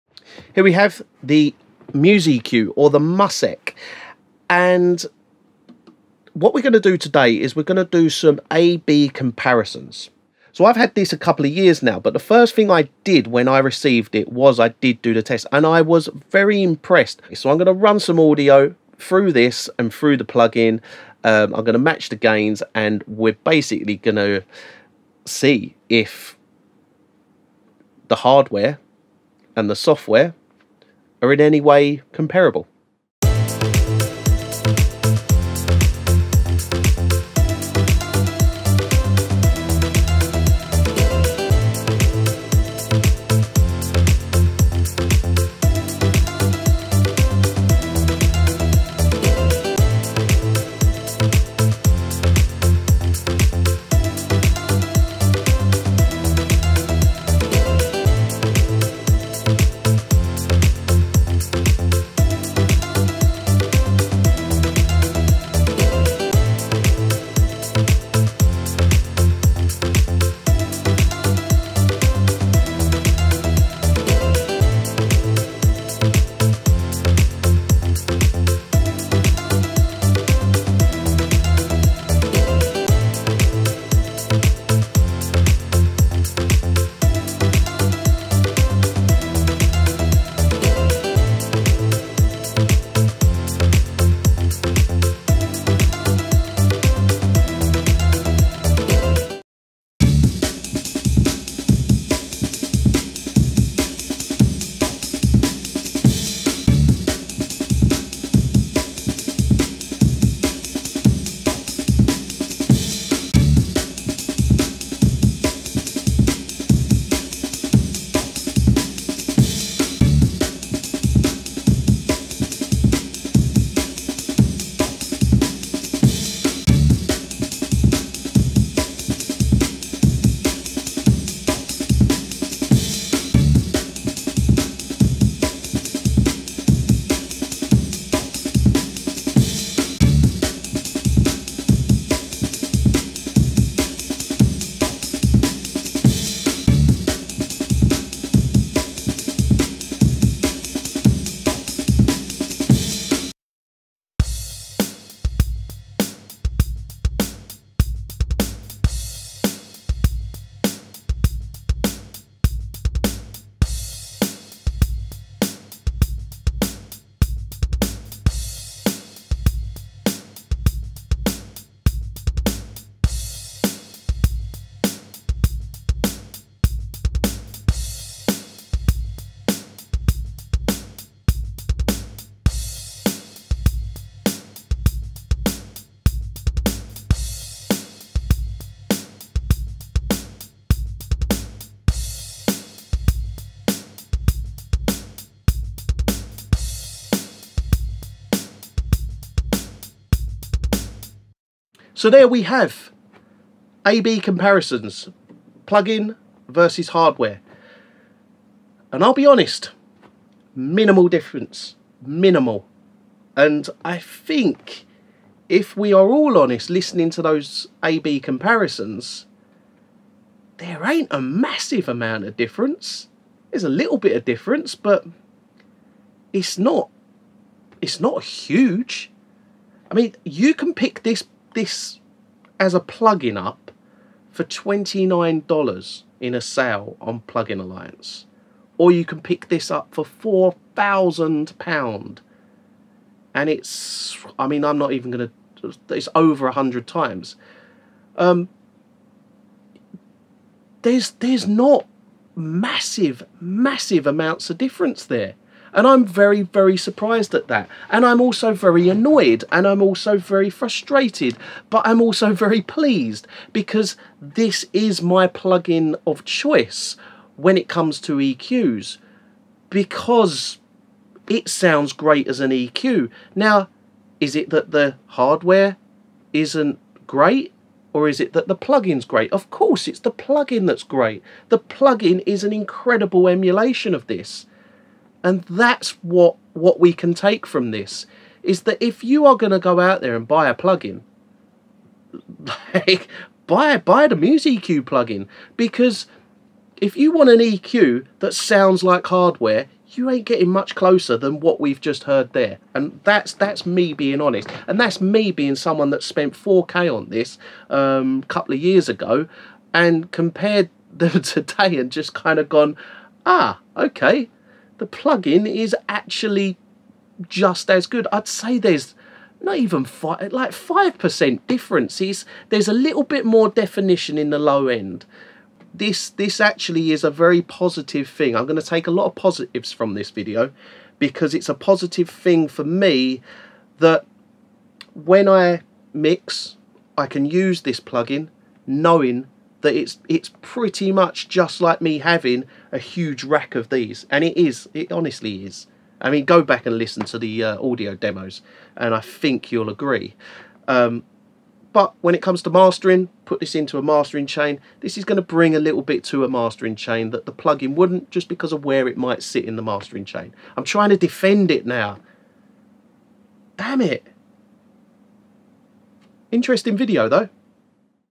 Elysia MusEQ Hardware VS Plugin Emulation
In this video I review how well the Elysia MusEQ plugin EQ compares to the analogue hardware. The Elysia MusEQ is a 5 band EQ with a beautiful sound to it. Let see who wins in this shootout of analogue vs digital.